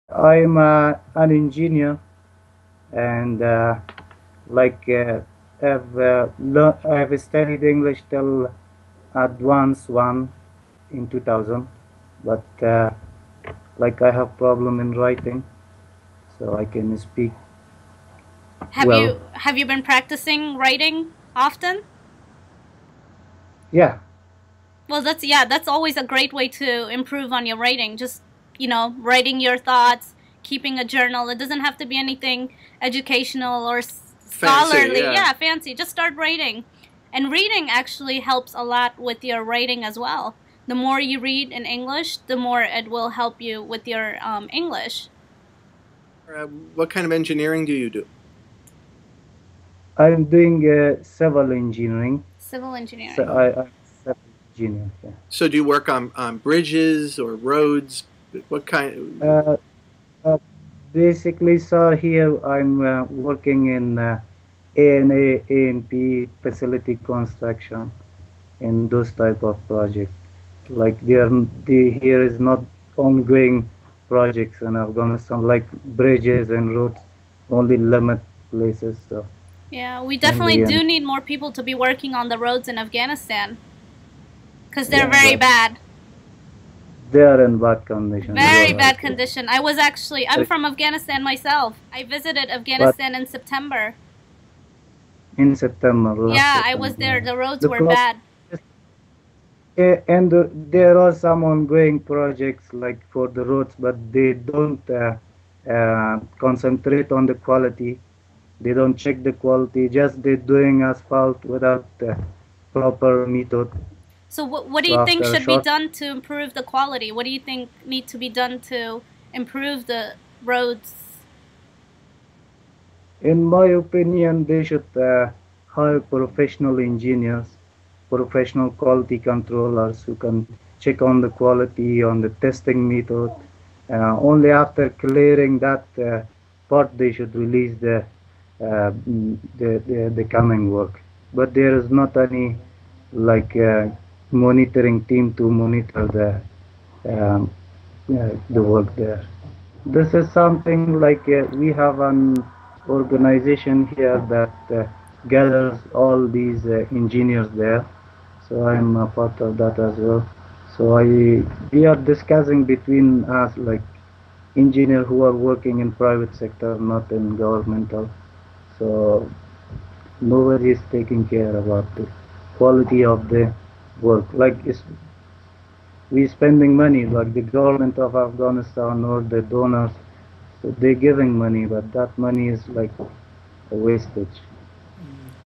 Afghanistan: Engineer Complains About Bad Roads
Conversational practice with Avi Arditti from VOA Learning English and Nargis Zalmai from VOA's Afghanistan Service.